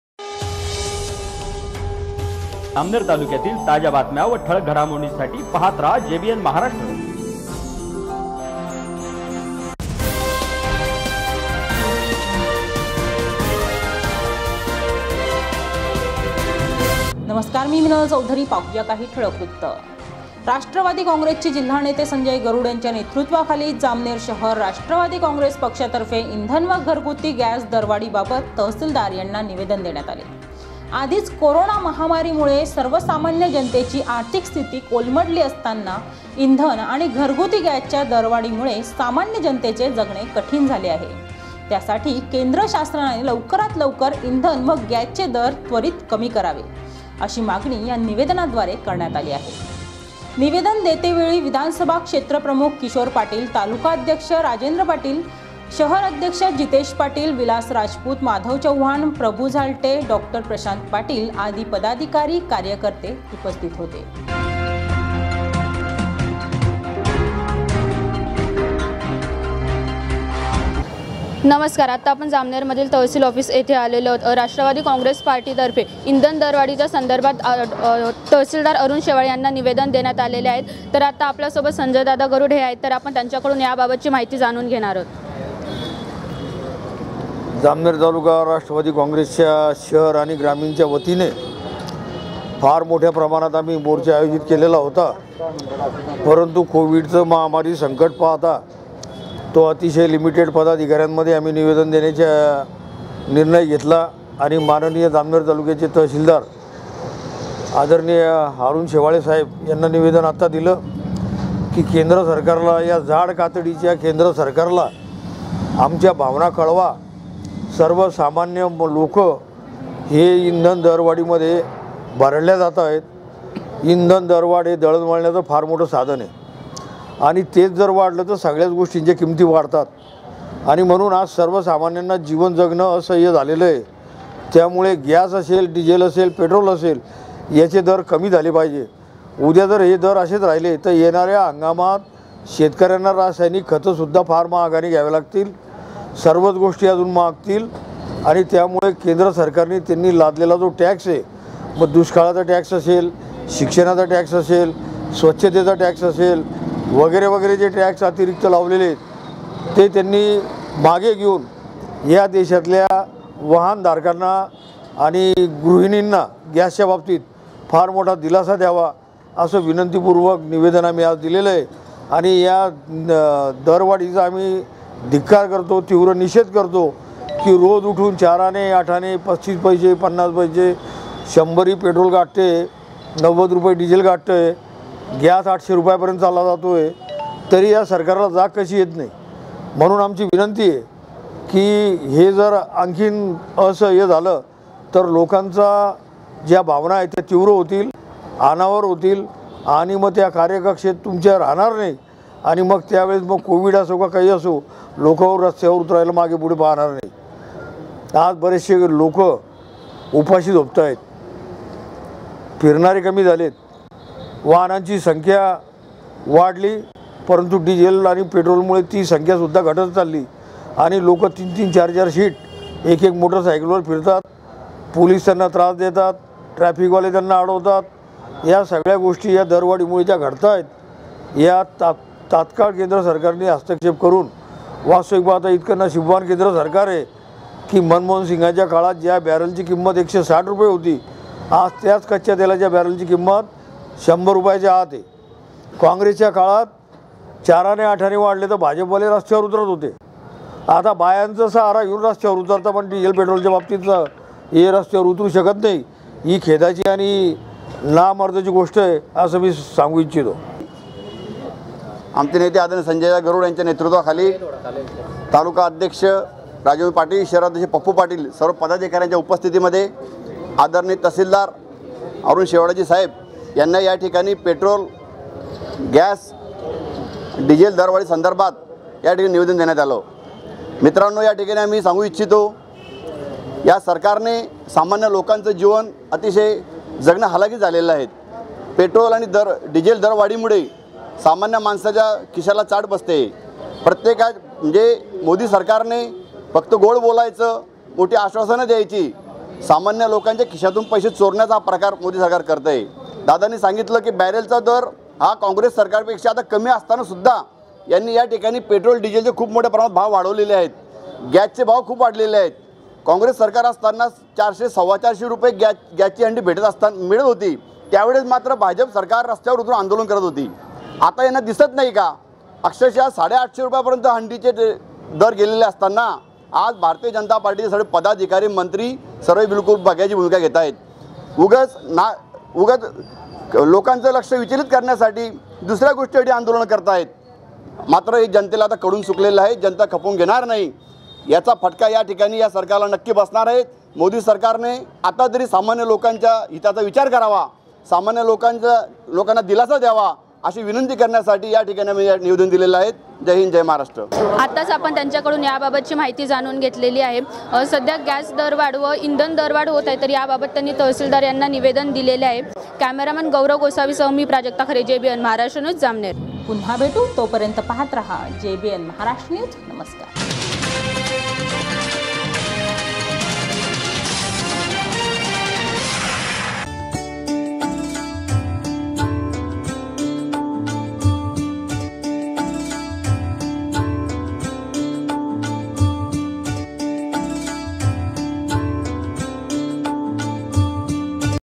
jbn maharashtra news jalgaon
राष्ट्रवादी काँग्रेस पक्षातर्फे इंधन व घरगुती गॅस दरवाढीबाबत तहसीलदार यांना निवेदन देण्यात आले.कोरोना महामारी मुळे सर्वसामान्य जनतेची आर्थिक स्थिती कोलमडली असताना इंधन आणि घरगुती गॅसच्या दरवाढीमुळे सामान्य जनतेचे जगणे कठीण झाले आहे.केंद्र शासनाने लवकरच दर कमी करण्याची मागणी निवेदनाद्वारे करण्यात आली.